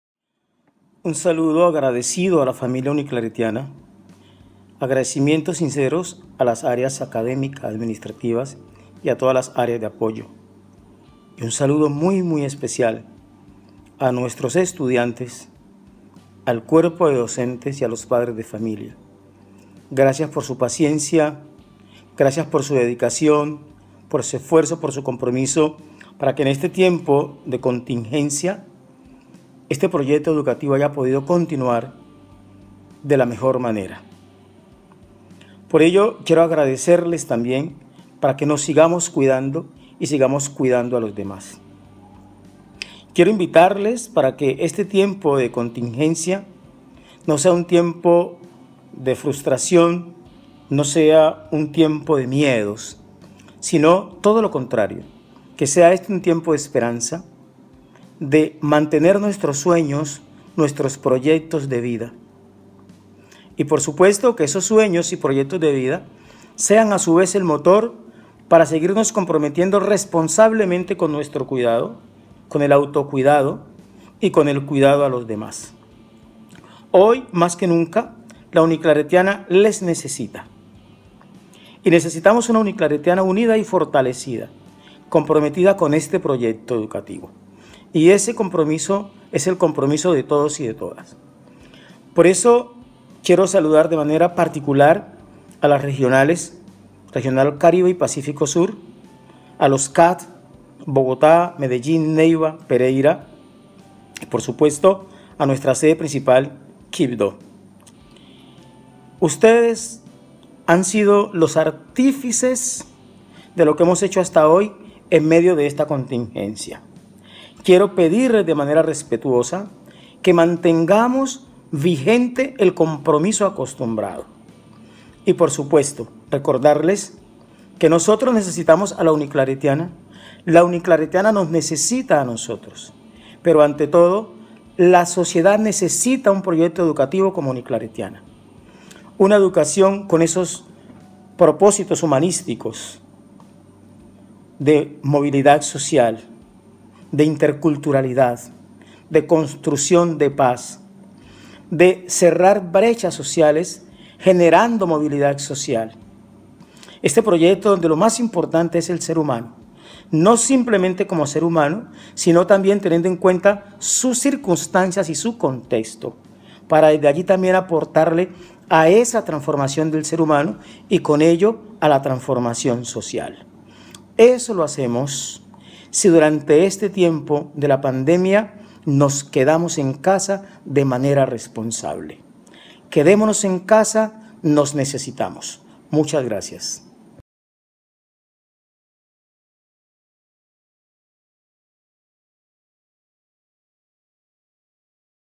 Mensaje del rector José Oscar Córdoba por la Cuarentena
Mensaje del rector José Oscar Córdoba a toda la comunidad universitaria.